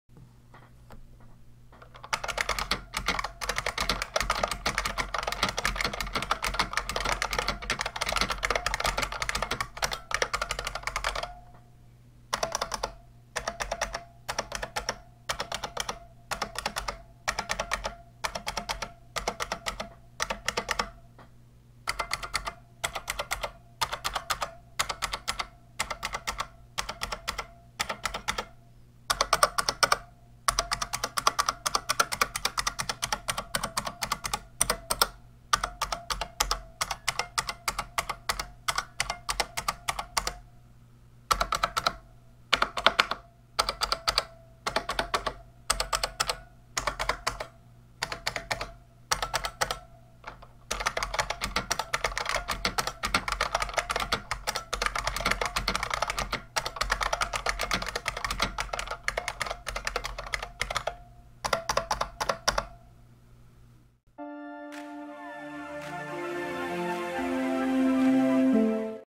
Typing sounds - 1990s IBM Model M
Specs:
Model M
IBM PS/2 Keyboard
Part Number 1391401
Buckling spring over membrane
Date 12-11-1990